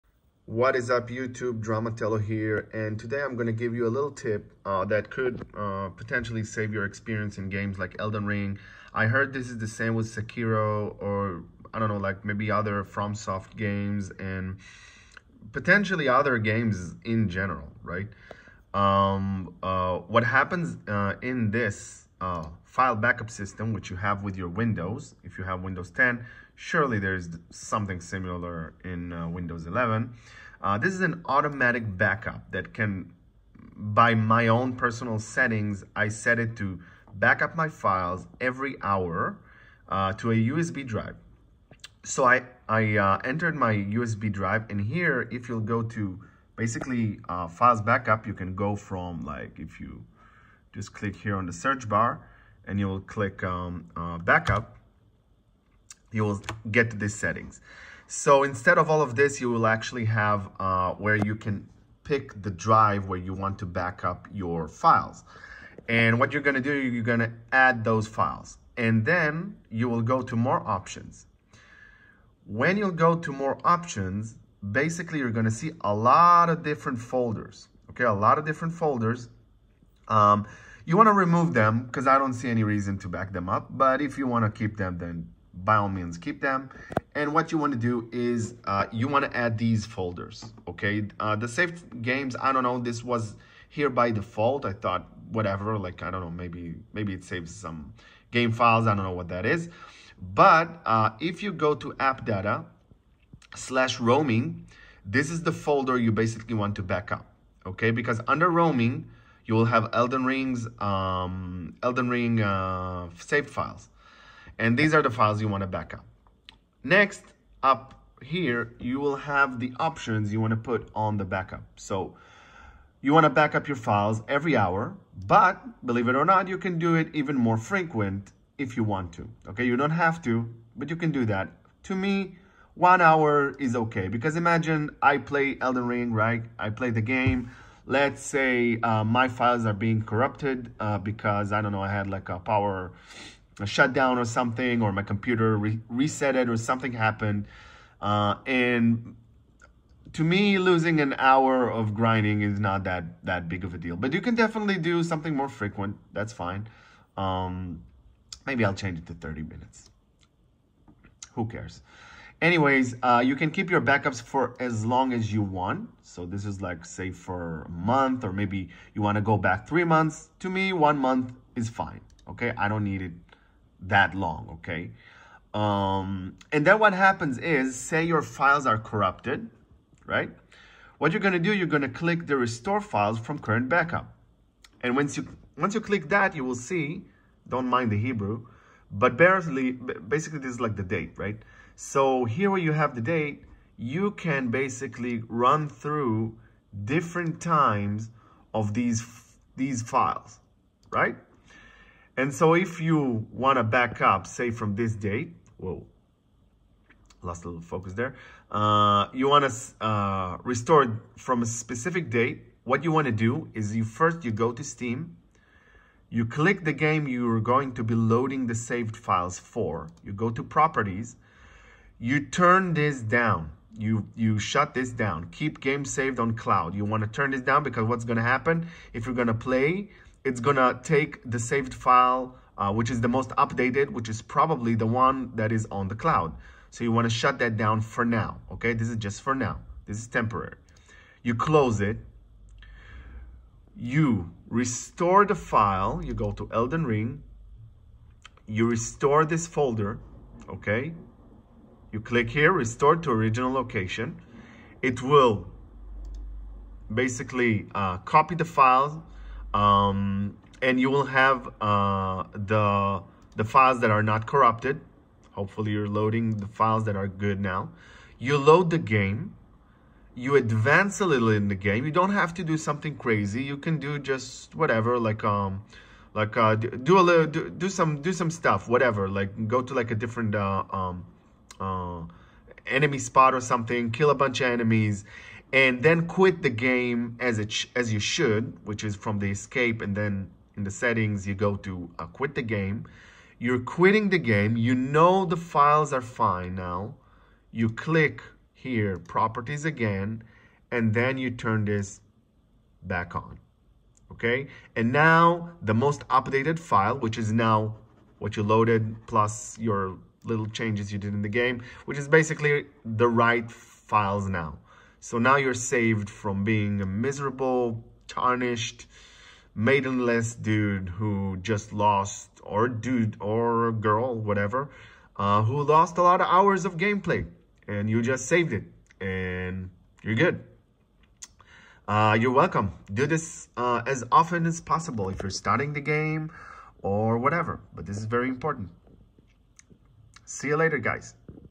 Automatically Backup Steam Saved Files For FREE! (No Software Needed)
This is a quick tutorial how to have constant backup of a folder containing saved files of games like Elden Ring to make sure that if you'll get corrupt saved data - you'll have saved files to come back to.